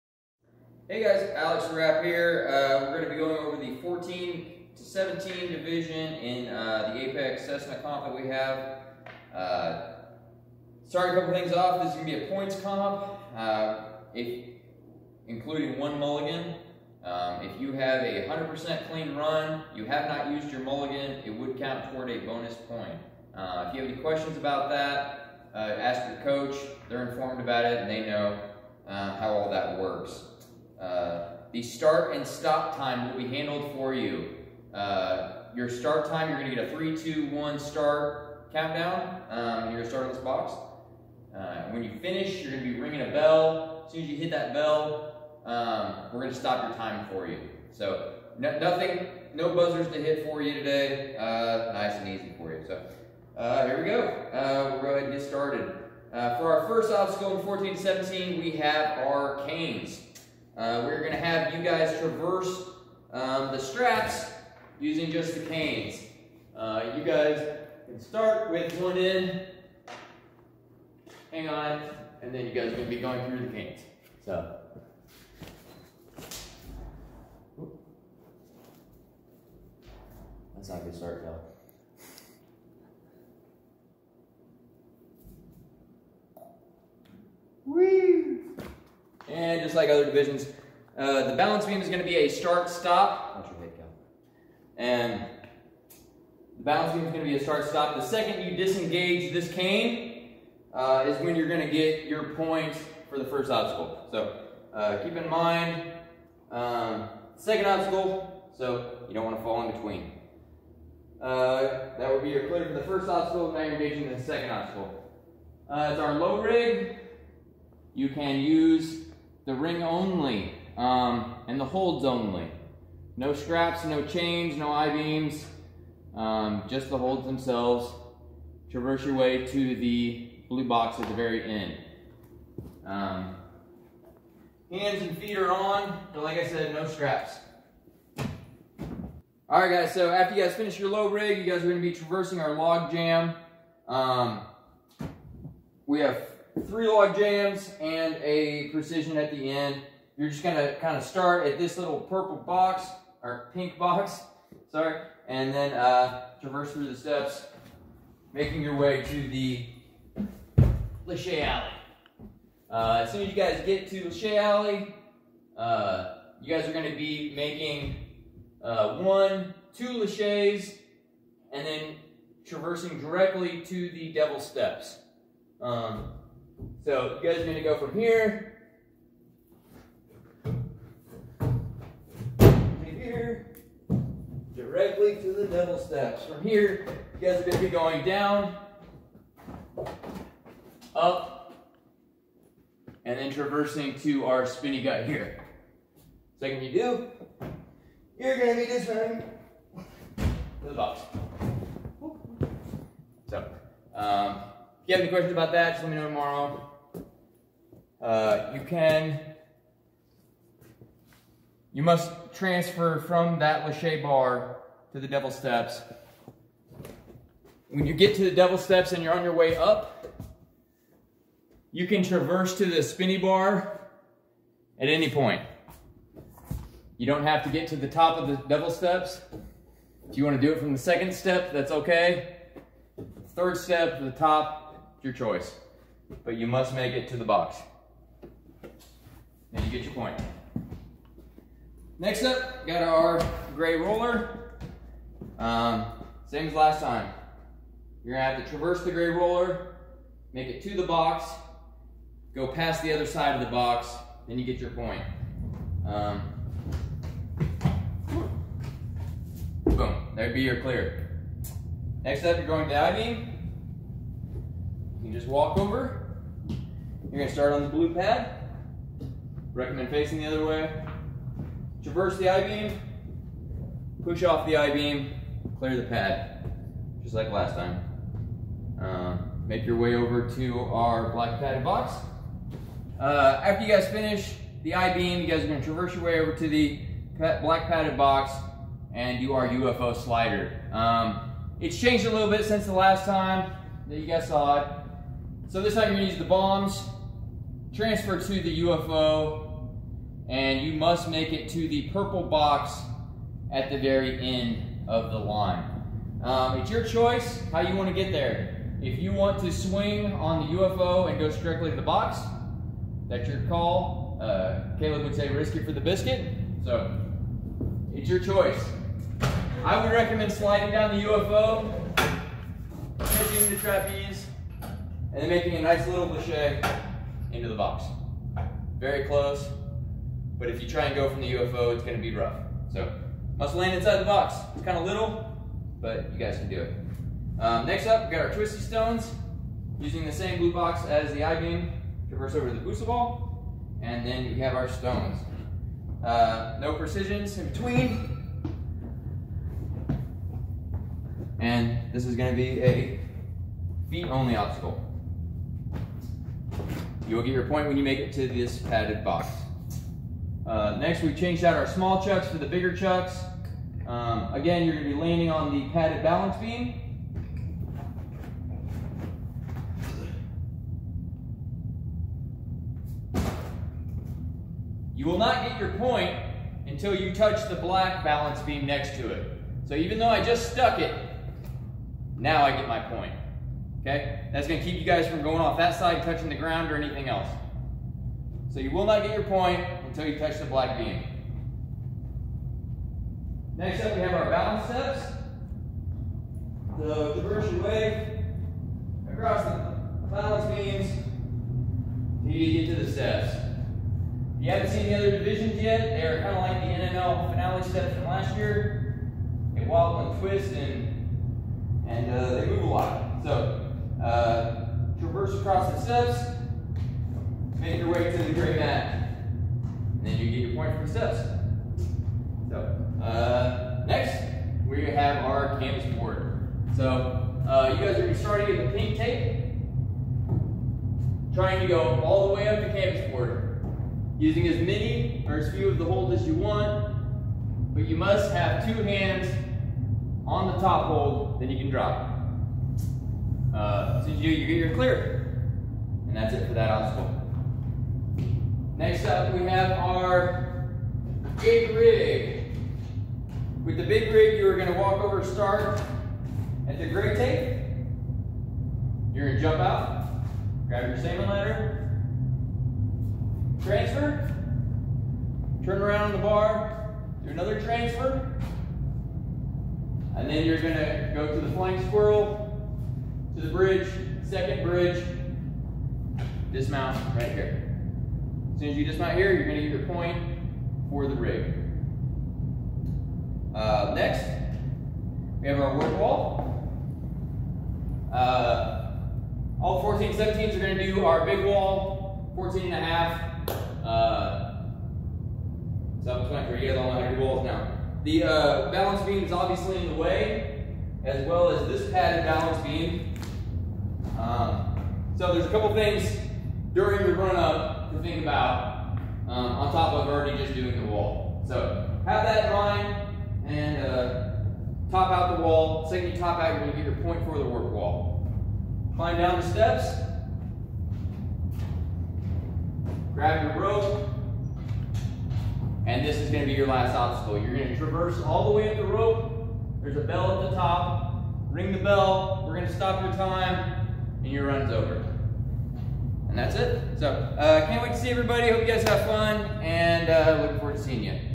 2021 APEX CSNA 14-17 Age Group Course Walkthrough
This is the 14-17 year old age group course walkthrough for the 2021 APEX CSNA competition.